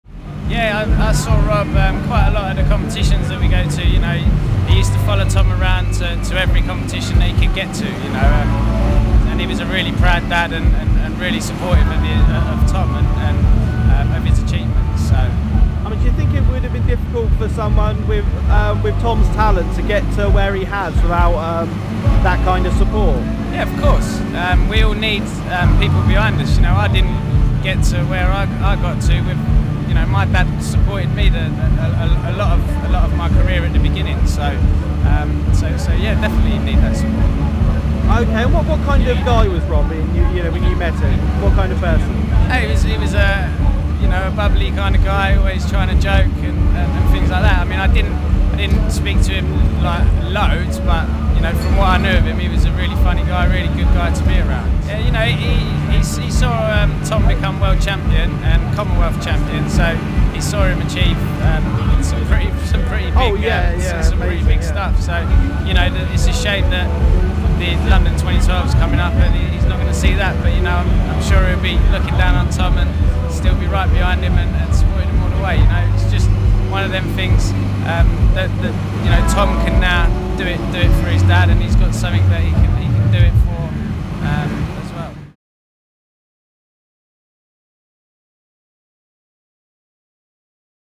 Diver's tribute to Rob Daley
Southampton's Pete Waterfield pays tribute to Tom Daley's father, who died from a brain tumour.